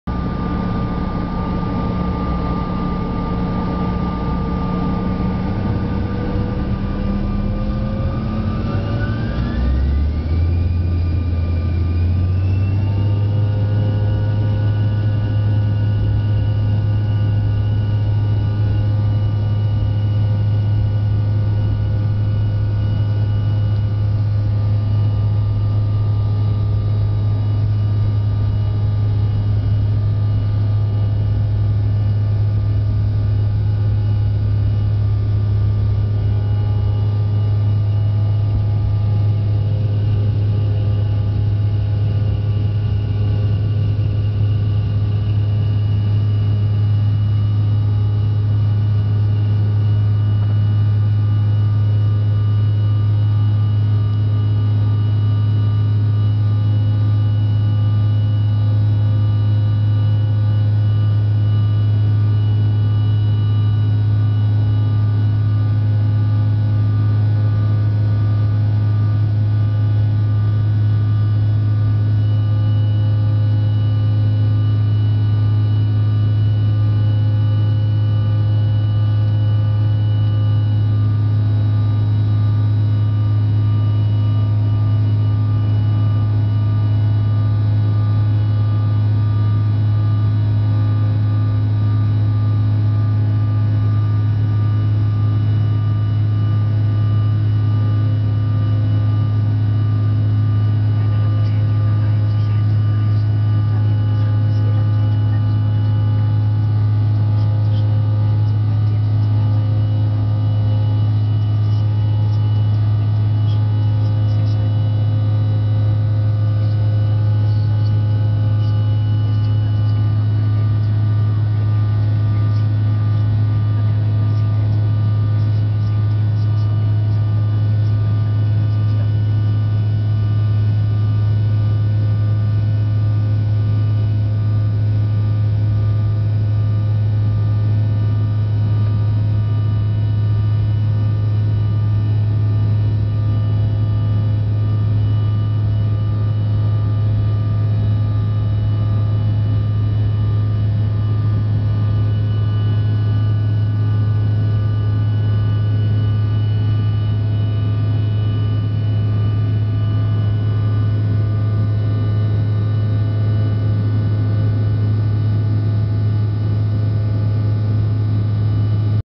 Birmingham BHX Airport take off
Bombardier CRJ900 Taking off from Birmingham BHX Airport going to Hamburg HAM